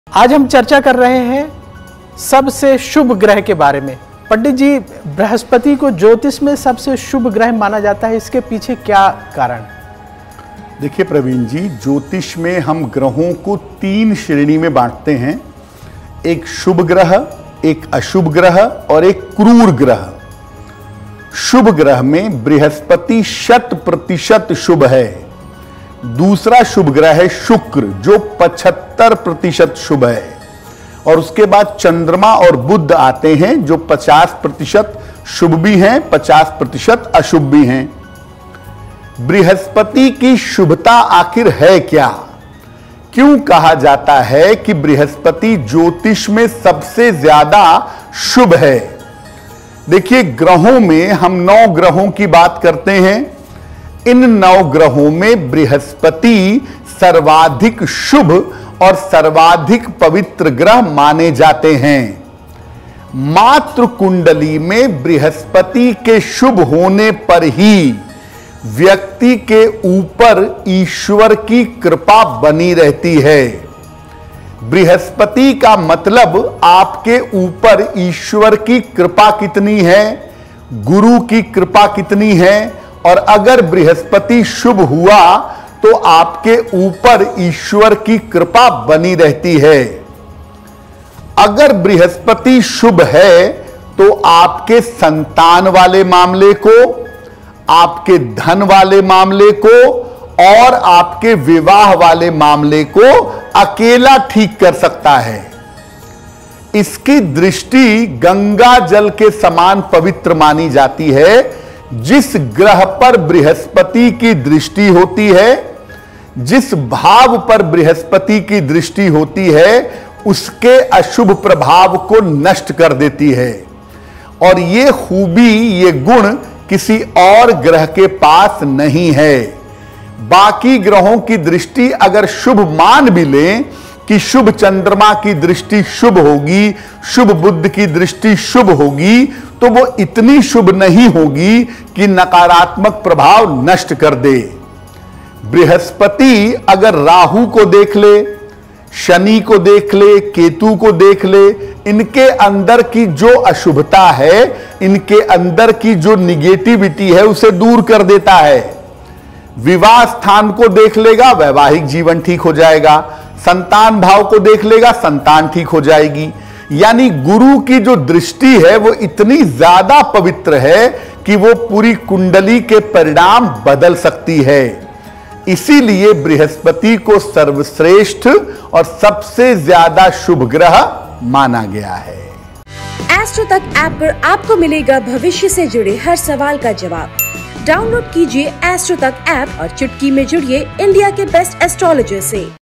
बृहस्पति की सुभता का महत्व, जानिए बृहस्पति की सुभता क्या है | Shailendra Pandey | AstroTak
नवग्रहों में बृहस्पति को गुरु और मंत्रणा का कारक माना जाता है...आइए ज्योतिर्विद शैलेंद्र पांडेय जी से जानते हैं, बृहस्पति की सुभता का महत्व और जानिए बृहस्पति की सुभता क्या है...